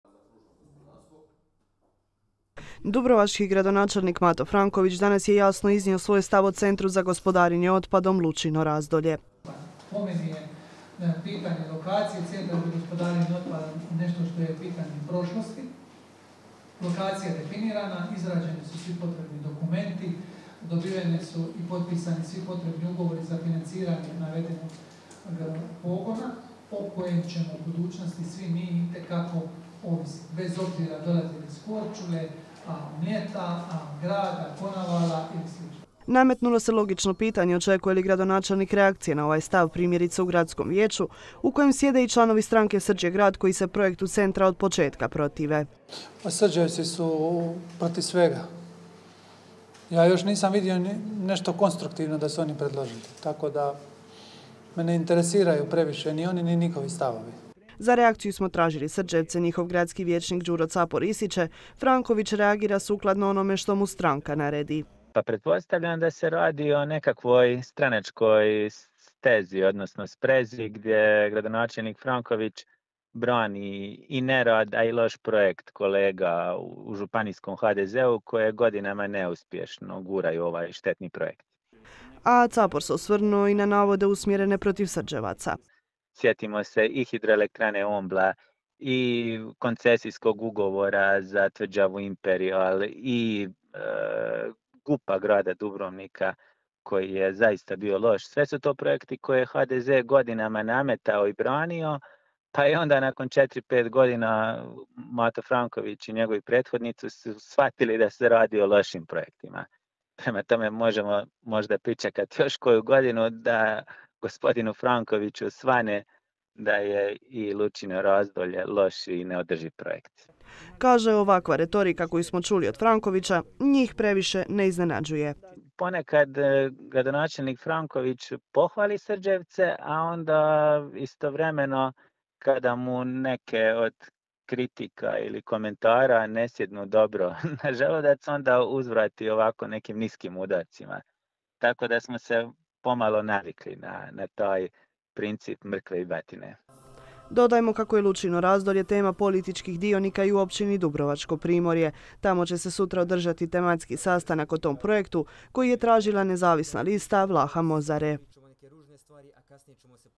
Zaiskrilo između Frankovića i Capora
A danas je zaiskrilo na relaciji Mato Franković-Srđevci, i to na temu gospodarenja otpadom Lučino razdolje. Franković je istaknuo kako podupire Lučino razdolje i kako su Srđevci uvijek protiv svega. Đuro Capor mu nije ostao dužan.